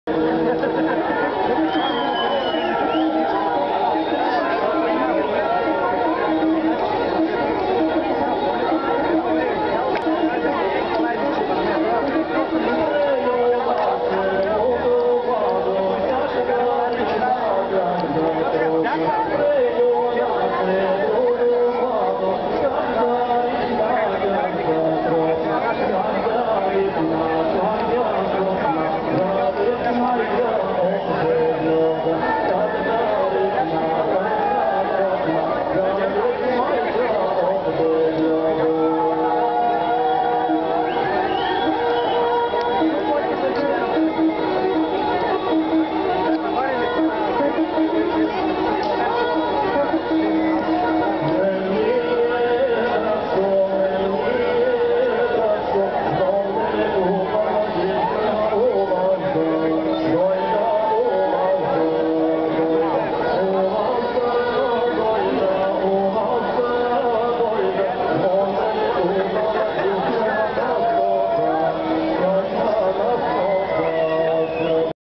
Gela 233.avi
Национално гайдарско надсвирване и събор в с. Гела Родопа планина